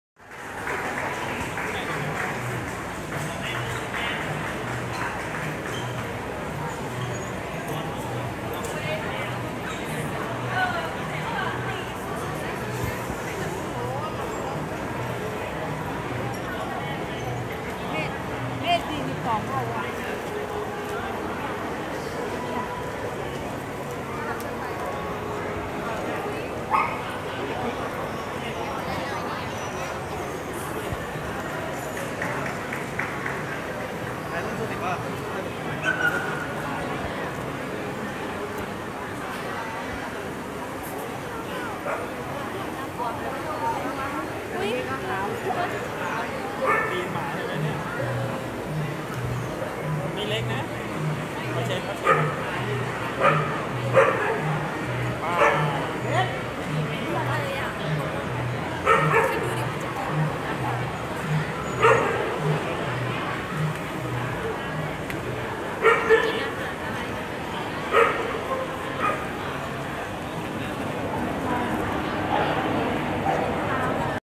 International Dog Show 2016 : インターナショナルドッグショー CentralPlaza Chaengwattana No.15
Best of Working Group judging from the 140th WKC Dog Show
International Dog Show 2016
インターナショナルドッグショー2016